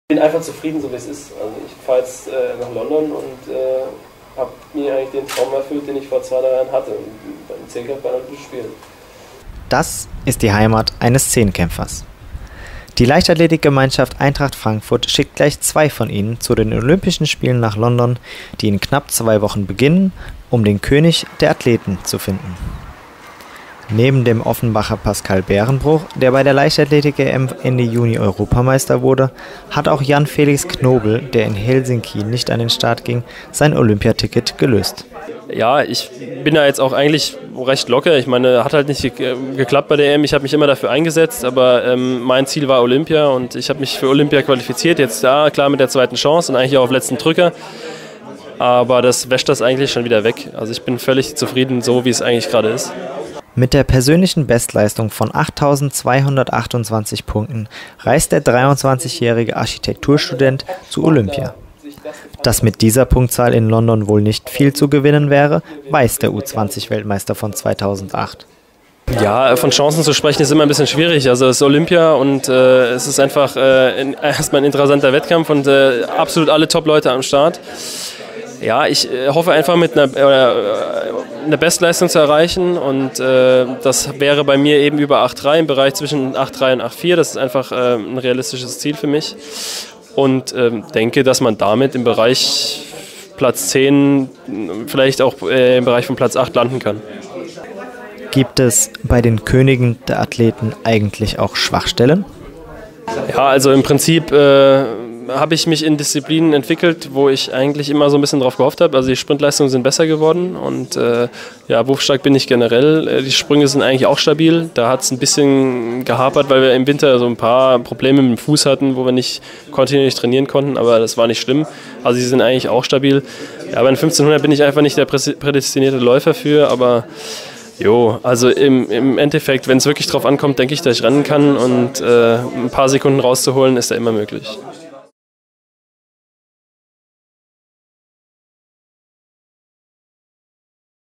Jan Felix Knobel bereit für Olympia 2012
Frankfurt - Zehnkämpfer Jan Felix Knobel von der LG Eintracht Frankfurt hat sich auf den letzten Drücker für Olympia 2012 qualifiziert. Der 23-jährige Architekturstudent reist mit einer persönlichen Bestleistung nach London.